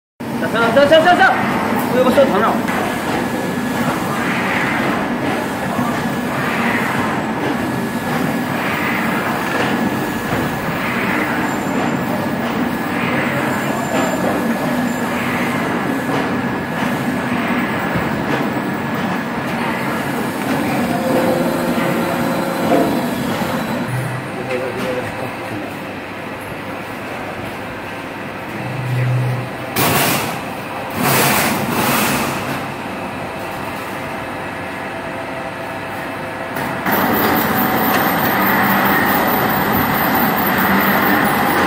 40 Mould Pizza Cone Making Machine Operating Video|How To Make Pizze Cone
This video show how pizza cone making machine works, and this machine can produce 40 pieces pizza cone one time. Following is the details information about 40 mould pizza cone making machine.